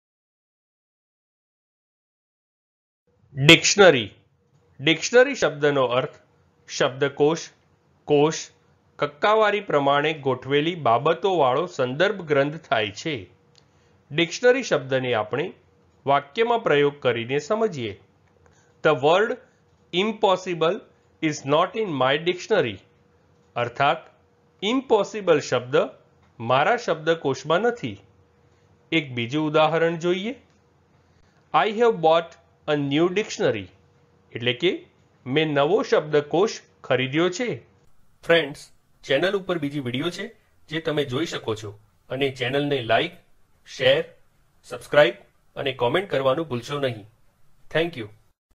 Dictionary Meaning in Gujarati | Dictionary નો અર્થ શું છે
આ વિડિયો માં તમે Dictionary નો Gujarati માં અર્થ સમજશો અને એની સાથે Dictionary નું pronunciation પણ શિખશો. એટ્લે કે તમે Dictionary ના meaning ની સાથે સાથે એ પણ શિખશો કે Dictionary ને કેવી રીતે બોલાય, Dictionary ને બોલવાની સાચી રીત કઈ છે.

તો બસ એક મિનટ માં શીખો Dictionary શબ્દ ને.

Dictionary ના આ examples થી સમજીએ

The word “Impossible” is not in my dictionary.

I have bought a new dictionary.